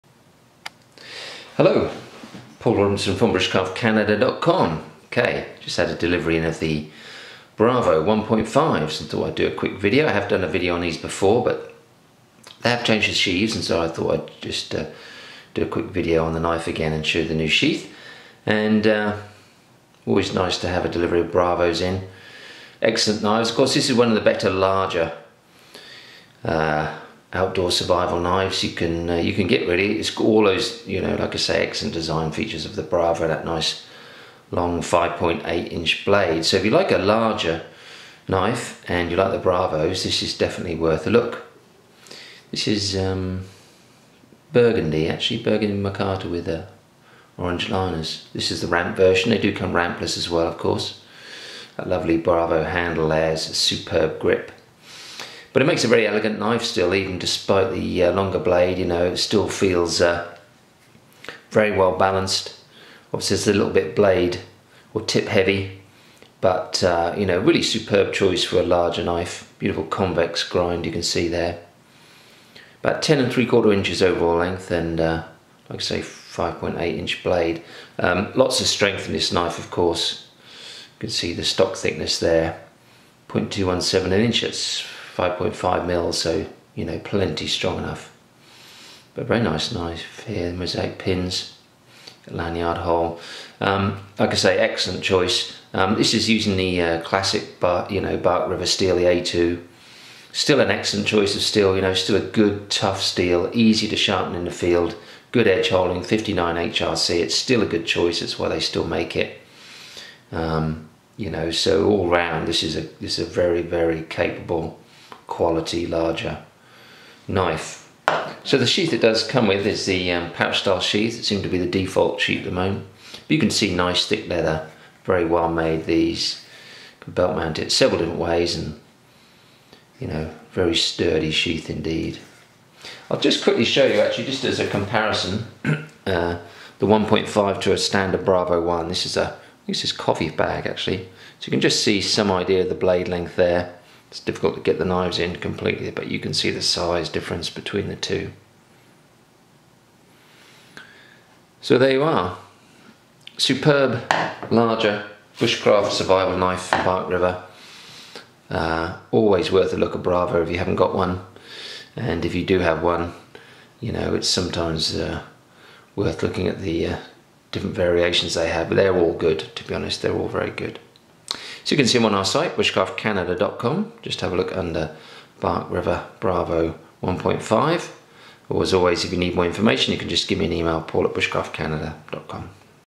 Barkriver Bravo 1 5 A2 view by www bushcraftcanada com
Just in a good range of Barkriver Bravo 1.5 in A2 Tool Steel, Ramped and Rampless.